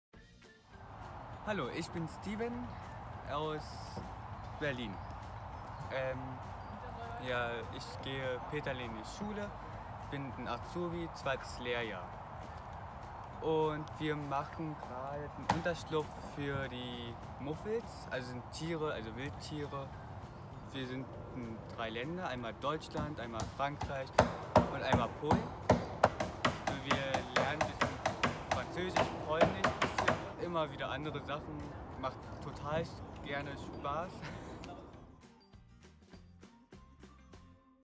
Die Stimme der Teilnehmenden #3
Eine Nachricht für euch von unseren Teilnehmenden! In dieser Videoserie werden die Teilnehmenden des deutsch-französisch-polnischen Austauschs über mehrere Wochen ihre Ideen und Gefühle mit euch teilen. Bleibt dran, um mehr zu erfahren!
Mehr zum Austausch '' Archi-Kultur '' auf unserem Blog:
In Partnerschaft mit Asso Gwennili ; ZSCKR Nowosielce ; @Lycée Kerbernez ; Peter-Lenné-Schule.
Unser Dank geht an das OFAJ DFJW.
Nos participant·e·s ont un message pour vous ! Dans cette série vidéos des participant·e·s de l’échange franco-germano-polonais vont partager leurs idées et ressentis sur plusieurs semaines. Restez à l’écoute pour en découvrir davantage !
Pour en savoir plus à propos de l’échange ‘’Archi-Kultur’’, rendez-vous sur notre blog :
En partenariat avec Gwennili, rencontres et échanges interculturels ; ZSCKR Nowosielce ; Lycée Kerbernez ; Peter-Lenné-Schule.
Nos remerciements à l' OFAJ DFJW.